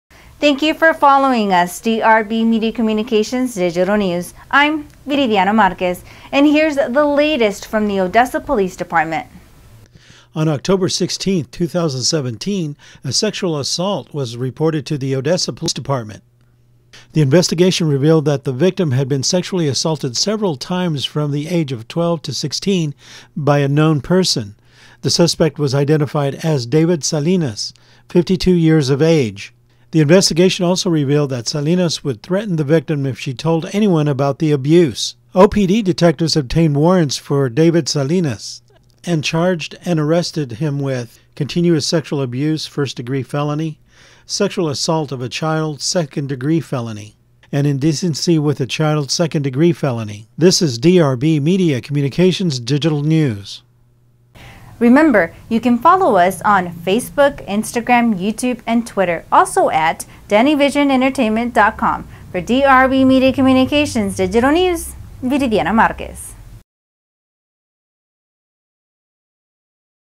NEWS BRIEF 110717 ODESSA POLICE CHARGE MAN IN CONNECTION TO SEXUAL ASSAULT
ODESSA, TEXAS/NOVEMBER 7, 2017/OPD - ON OCTOBER 16, 2017, A SEXUAL ASSAULT WAS REPORTED TO THE ODESSA POLICE DEPARTMENT. THE INVESTIGATION REVEALED THAT THE VICTIM HAD BEEN SEXUALLY ASSAULTED SEVERAL TIMES FROM THE AGE OF 12 TO 16 BY A KNOW PERSON. THE SUSPECT WAS IDENTIFIED AS DAVID SALINAS, 52-YEARS-OF-AGE.
THE INVESTIGATION ALSO REVEALED THAT SALINAS WOULD THREATEN THE VICTIM IF SHE TOLD ANYONE ABOUT THE ABUSE.
OPD DETECTIVES OBTAINED WARRANTS FOR DAVID SALINAS, AND CHARGED AND ARRESTED HIM WITH CONTINUOUS SEXUAL ABUSE “1ST DEGREE FELONY”, SEXUAL ASSAULT OF A CHILD “2ND DEGREE FELONY”, AND INDECENCY WITH A CHILD “2ND DEGREE FELONY.”